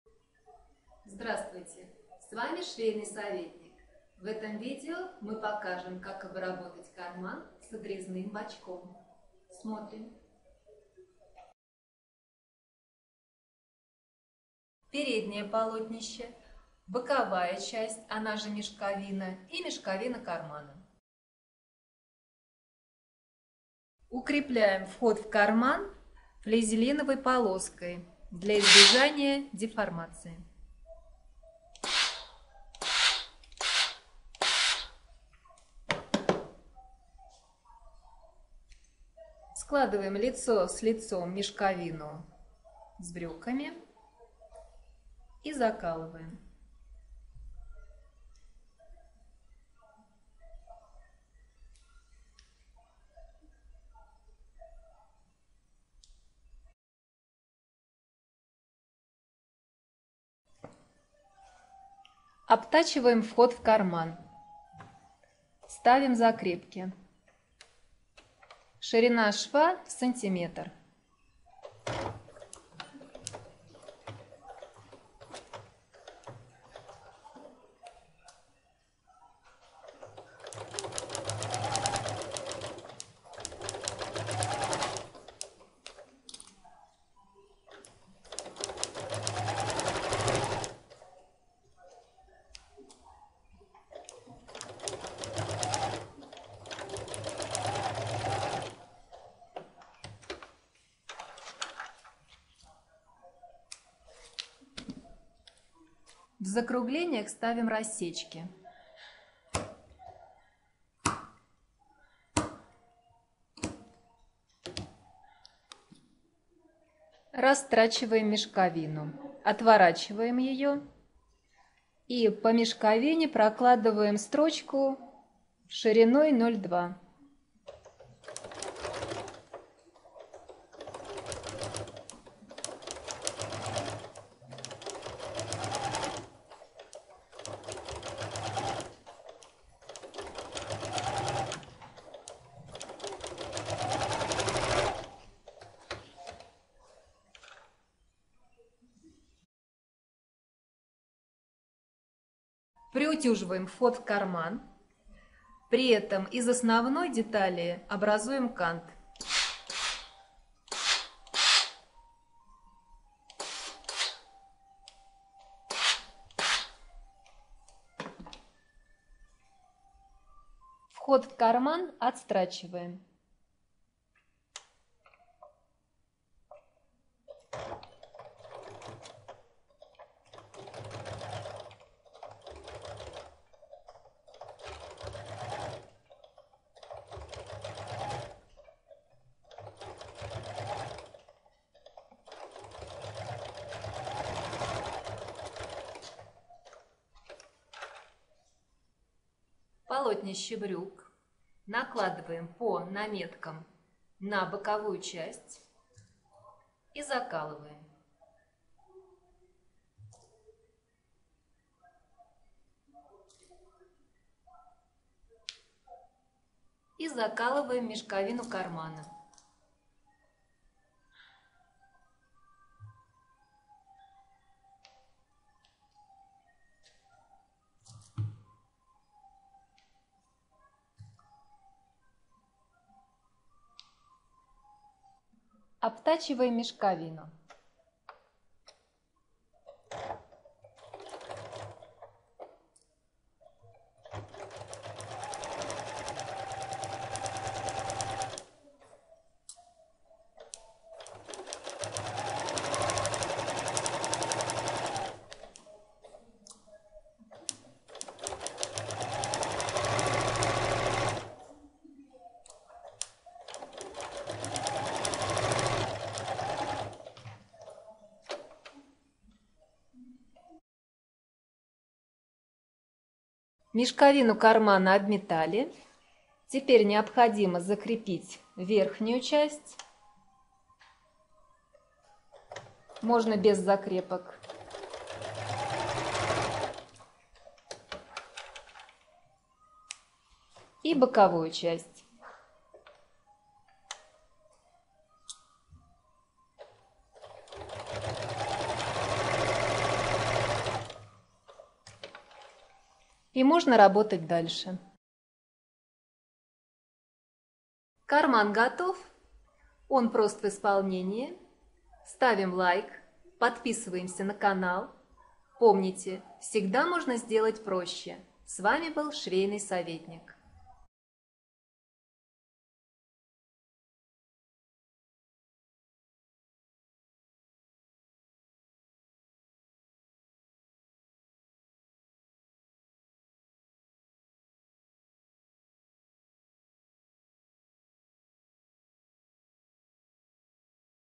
Карман с отрезными бочком: сшить просто!
Популярный карман для юбок и брюк! Шьется очень просто. Используются разные ткани и различная форма переднего среза. В видео подробно показано пошаговое изготовление. Размер кармана должен соответствовать размеру руки.

Присоединяйтесь, добавляйтесь в друзья!